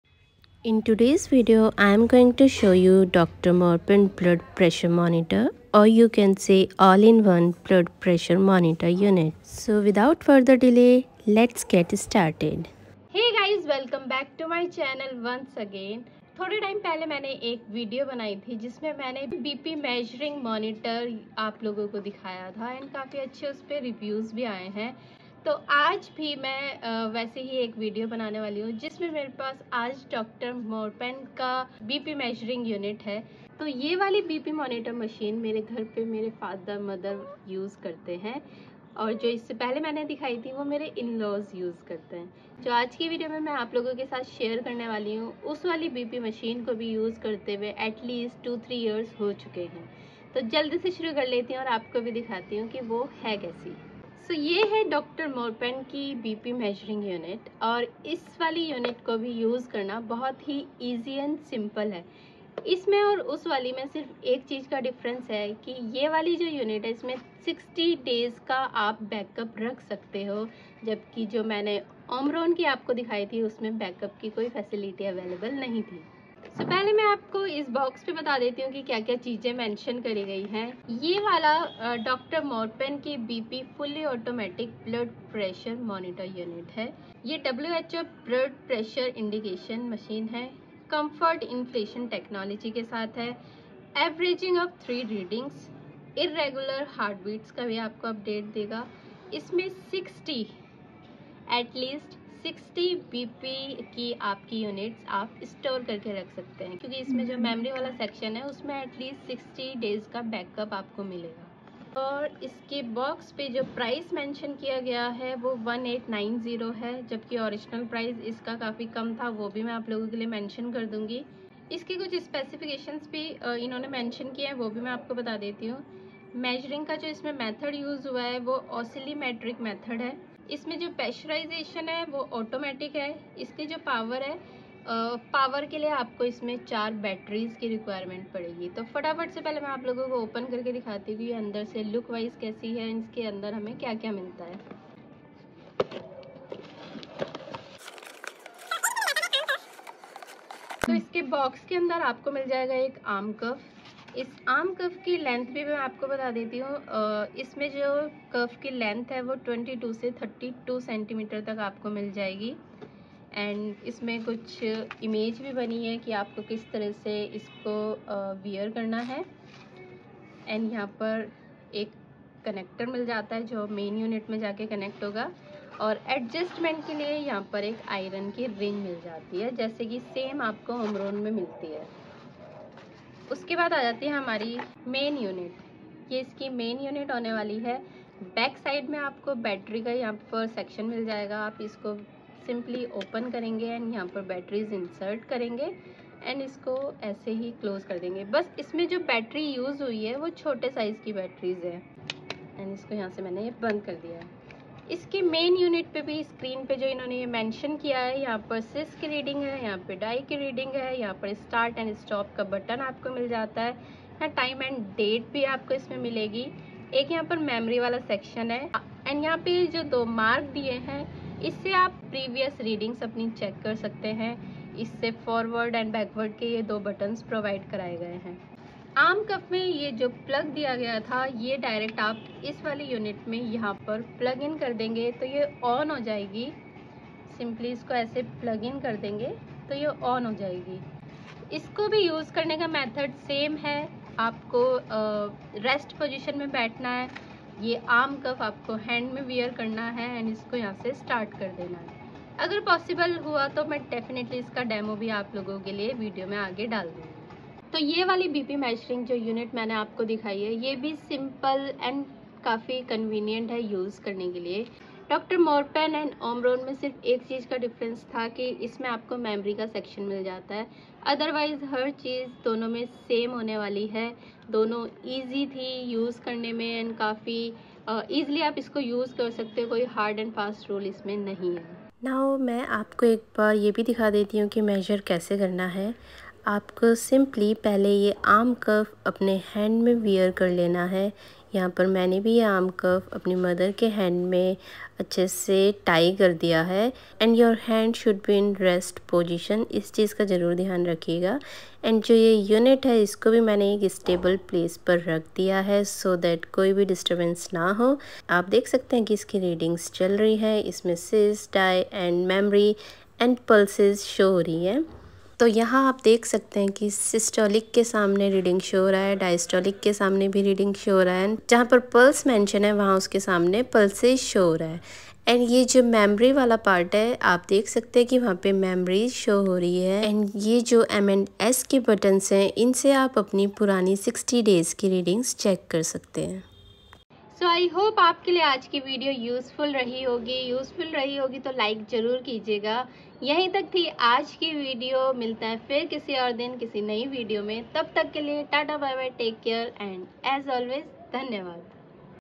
Dr Morepen BLOOD PRESSURE Monitor 🔥 BEST BP Machine In India 2024 🔥 घर बैठे अपना बीपी कैसे नाप्पे 🔥
🩺 Are you curious about how to effectively monitor your blood pressure at home? Look no further! In this comprehensive video, we dive into the world of BP monitoring machines and provide practical use as well.
📈 Understanding and managing your blood pressure is crucial for maintaining overall health and well-being. With the rise of at-home monitoring devices, it's easier than ever to keep track of your BP levels and make informed decisions about your health.
🔍 In this video, we'll walk you through everything you need to know about BP monitoring machines, from how they work to how to use them correctly. Our step-by-step demonstrations will guide you through the process.
And if you have any questions or experiences to share about BP monitoring machines, we'd love to hear from you in the comments below!

GEAR USED:-
Mobile Camera
Secondary Mobile
Mobile Wireless Mic
Directional Mic
Lavalier Lapel Microphone